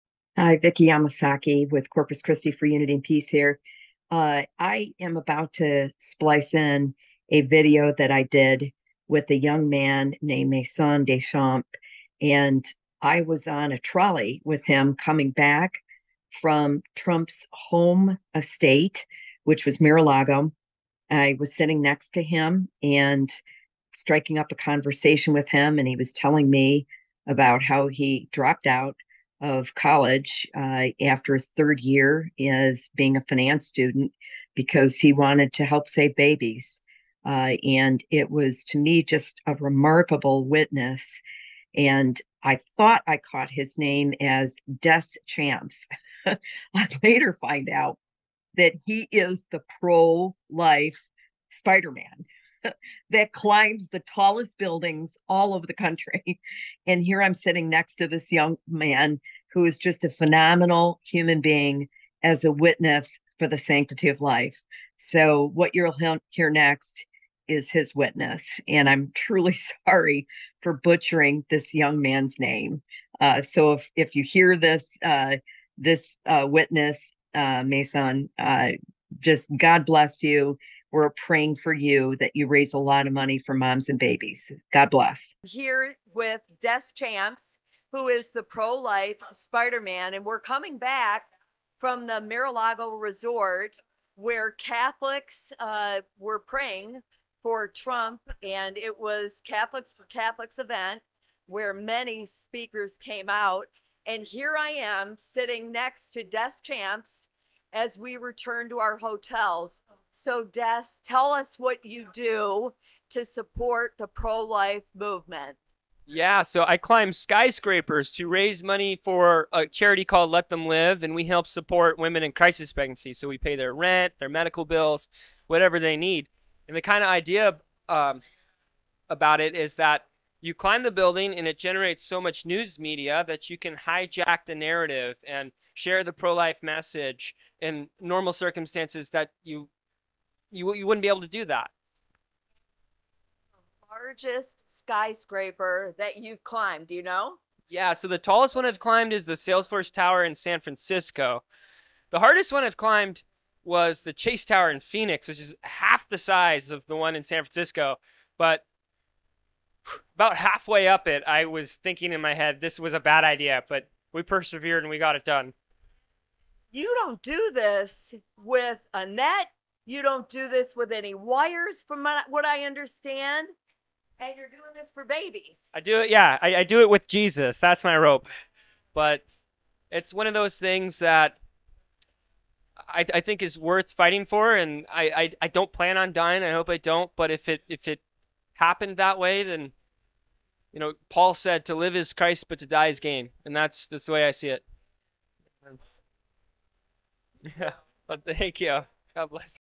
Prolife Spiderman Maison DesChamps
Vicki Yamasaki encountered Maison Des Champs, also known as the “Pro-Life Spider-Man,” returning from the Catholics for Trump prayer event at Mar-A-Lago on the Feast of St. Joseph.  This daring climber has scaled the Las Vegas Sphere, the Sales Force Tower in San Francisco, the Accenture Tower in Chicago and more (all free-climbing) ... doing this for moms and babies.